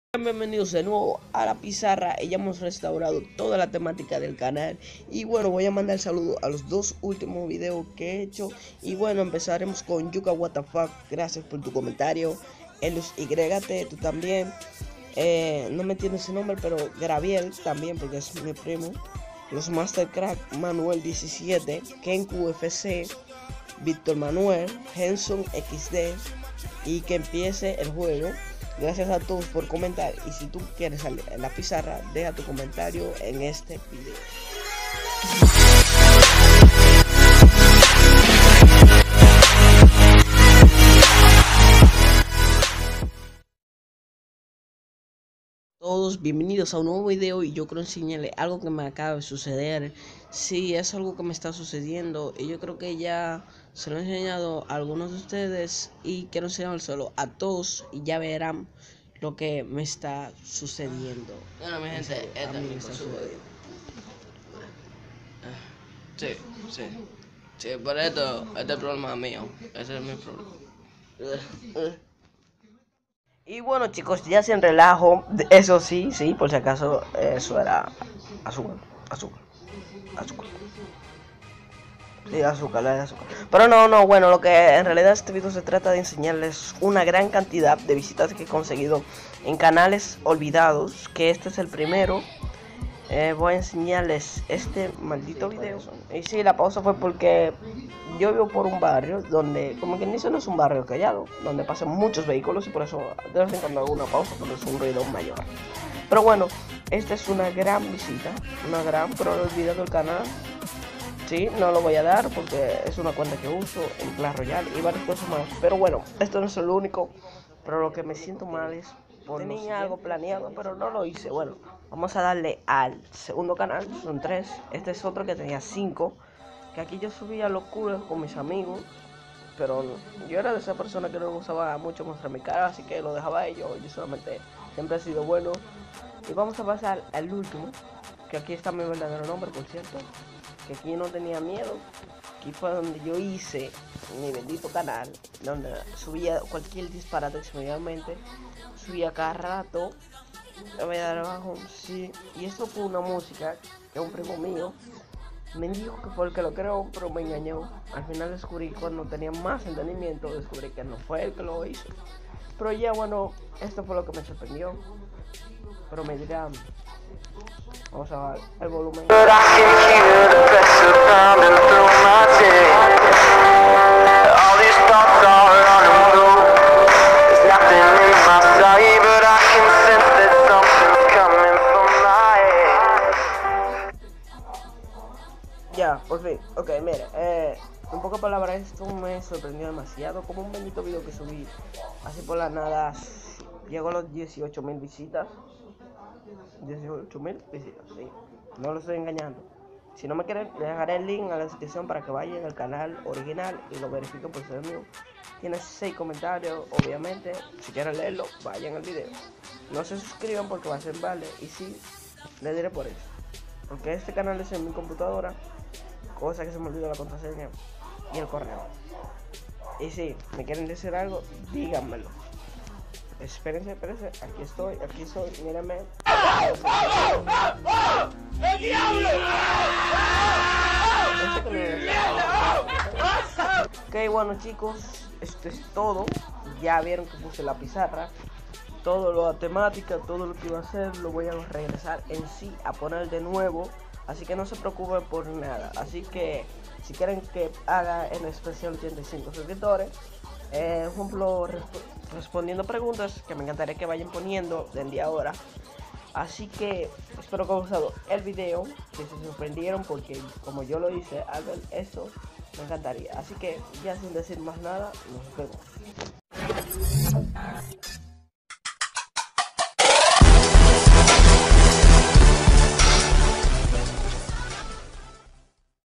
Canales que superan al mio en visitas | vuelve la pizarra
Este vídeo se trata de lo que me paso que fui a ver mi cuenta i encontré tres canales mios que olvide y tiene una gran cantidad de visitas

Dejarme tu like y un comentario gracias

Dime cual seria el próximo vídeo